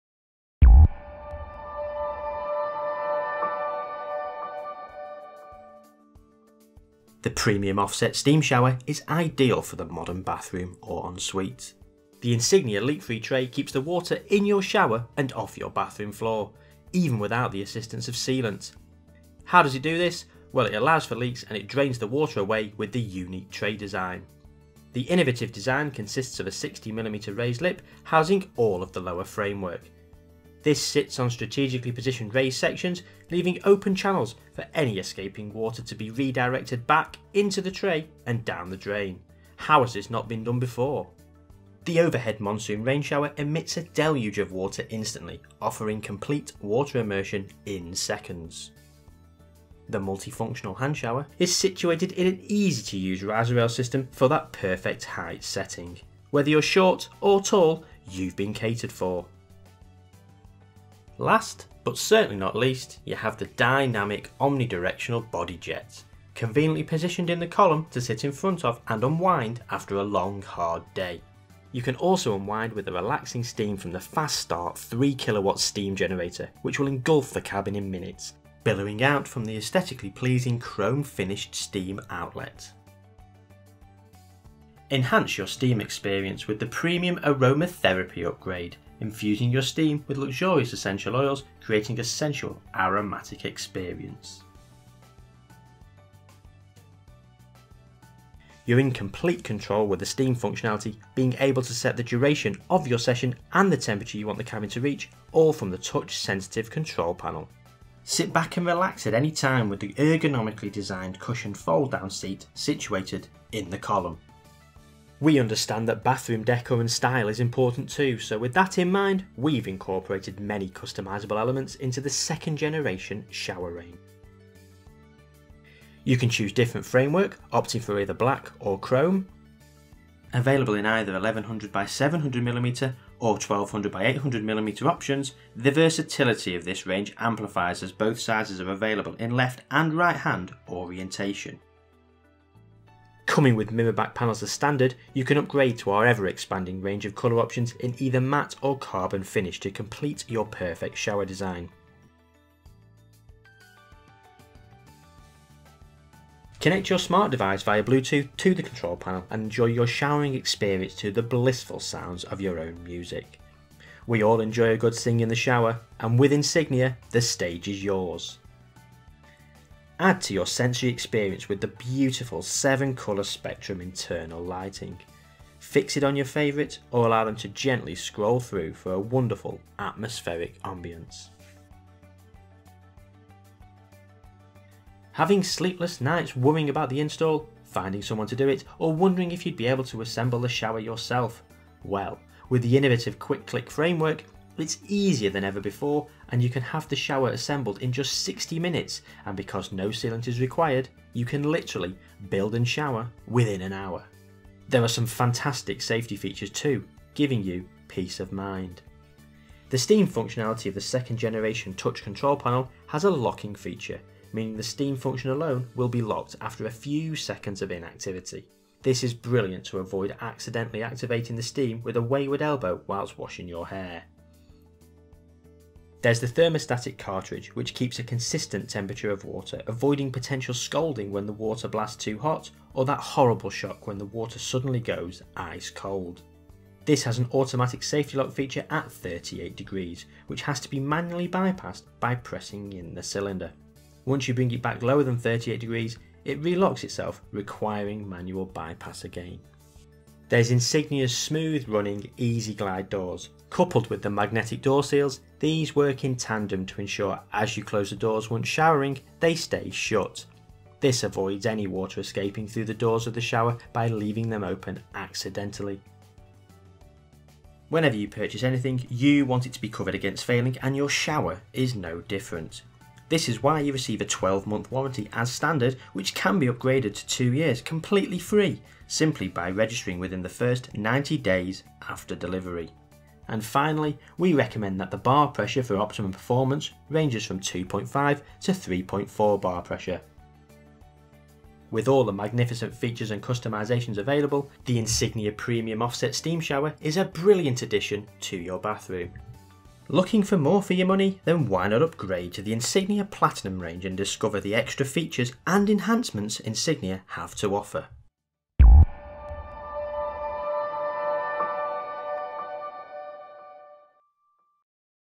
Insignia Premium Offset Steam Shower - 2nd Generation Showers
Welcome to Insignia's 2nd Generation of showers. In this video we cover the Insignia Premium Offset Steam Shower. For more information on this, or any of the showers within the range please

Insignia's 2nd Generation Showers boast a plethora of new features. Most notably, the industry changing 'LeakFree' tray. The unique tray allows escaping water to flow back into the tray through allotted channels and draining away, leaving your bathroom floor dry. No sealant is required either, the shower is designed to leak with the unique design.

There's the QuickClick framework which makes installation of the shower easier than ever before. Simply clicking together the frame and using wing nuts for the back panels and column, you then screw everything together. All this can be achieved in 60 minutes and because no sealant is required, you can build and shower - within an hour.

Another key feature within the range is customisation. Available in two sizes, 1100mm x 700mm or 1200mm x 800mm, the variations don't stop there. Choose you framework colour of either black or chrome. You can also choose the orientation of the shower cabin as well, whether you want it to be a left hand, or right hand enclosure. As with all Insignia Showers you can opt for one of our internal colour finishes as well, choosing any of our matte or carbon colour options.

Packed with trademark Insignia features too, you have the Bluetooth connectivity to listen to your own music inside the shower enclosure. Multi-coloured Chromotherapy lighting to set the and relax after a long hard day.